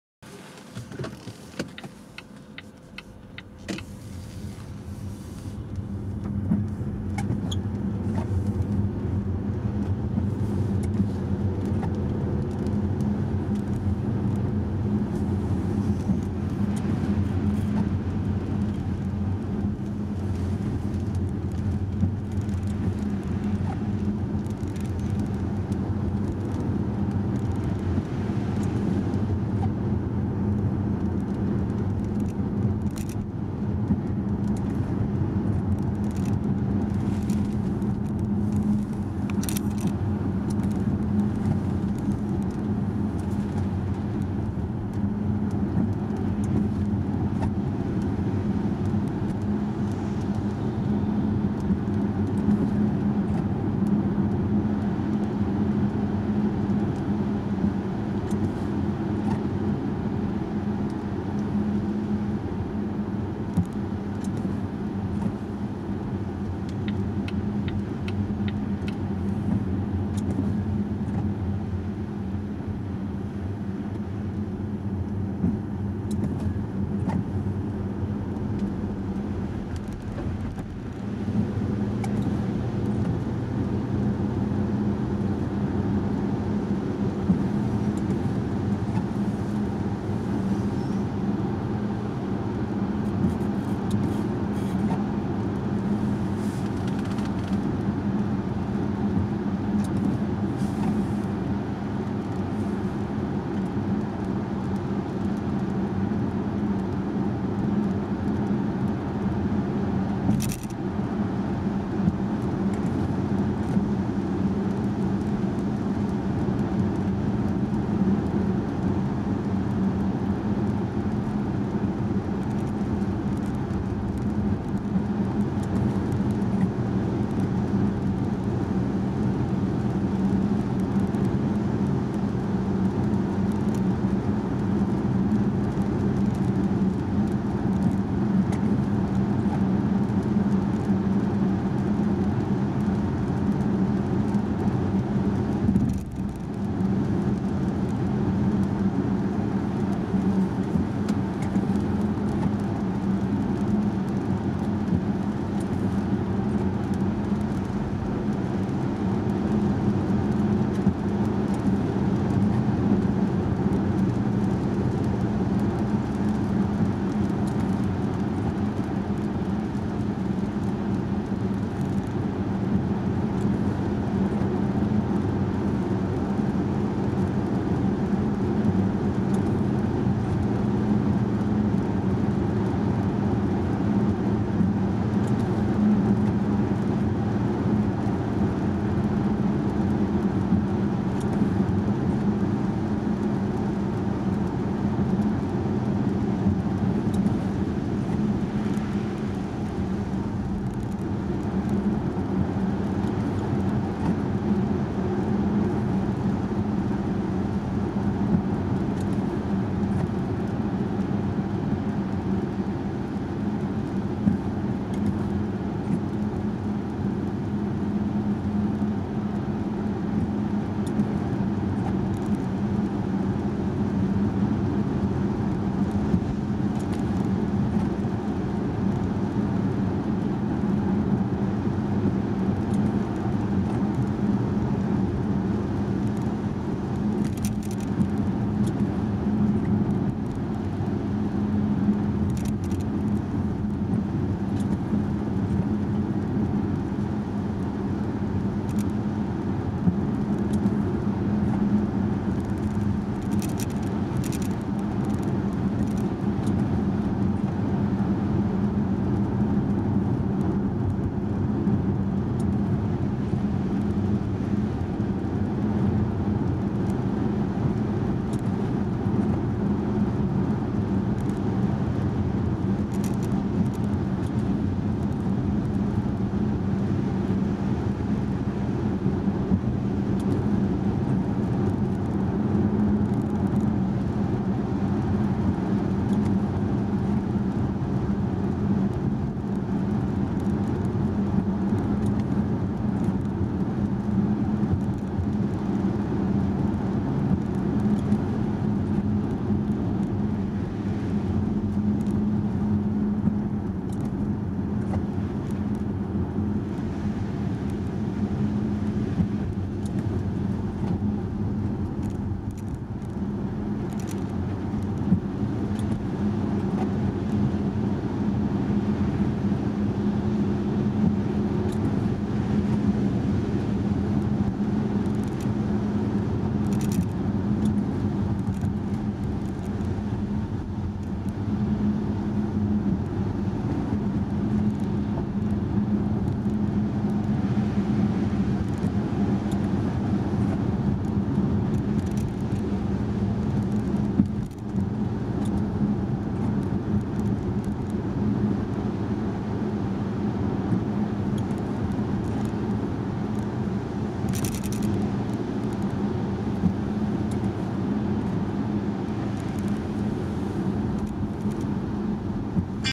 Driving in Rain, Night, Lahti FINLAND - September 15, 2024
Tämä on ilmainen ja tekijänoikeusvapaa video. NIIN AUTAT TÄTÄ KANAVAA!

Then you help me to make more videos for you!

time of the recording: September 15, 2024 - started at 02:05 a.m.
place: (starting from Renkomäki) street Uudenmaankatu, city of Lahti, Finland
car: Volvo S90